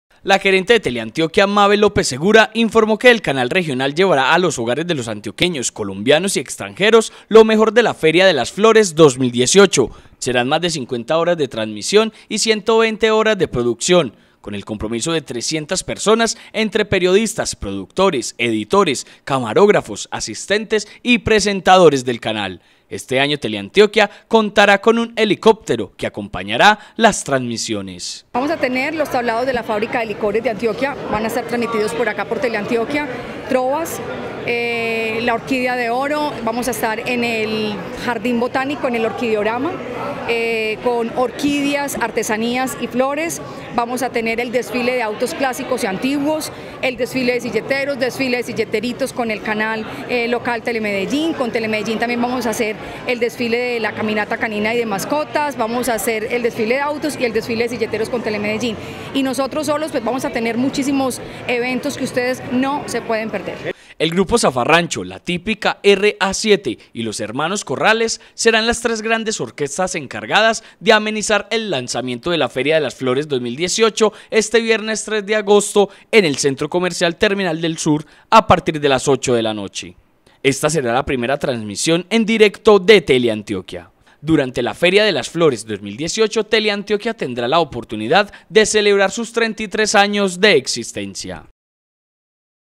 TELEANTIOQUIA DE FLORES 2018
El Canal Regional Teleantioquia se sigue consolidando como el "Canal de la Flores". Con una inversión de 300 millones de pesos, realizará más 24 transmisiones durante la Feria de la Flores 2018.

La gerente de Teleantioquia, Mábel López Segura, informó que el Canal Regional llevará a los hogares de los antioqueños, colombianos y extranjeros lo mejor de la Feria de las Flores 2018. Serán más de 50 horas de transmisión y 120 horas de producción, con el compromiso de 300 personas entre periodistas, productores, editores, camarógrafos, asistentes y presentadores del Canal. Este año, Teleantioquia contará con un helicóptero que acompañará las transmisiones.

El Grupo Zafarrancho, la Típica R.A.7 y los Hermanos Corrales serán las tres grandes orquestas encargadas de amenizar el lanzamiento de la Feria de las Flores 2018, este viernes, 3 de agosto, en el Centro Comercial Terminal del Sur a partir de las 8 de la noche. Ésta será la primera transmisión en directo de Teleantioquia.

Durante la Feria de la Flores 2018, Teleantioquia tendrá la oportunidad de celebrar sus 33 años de existencia.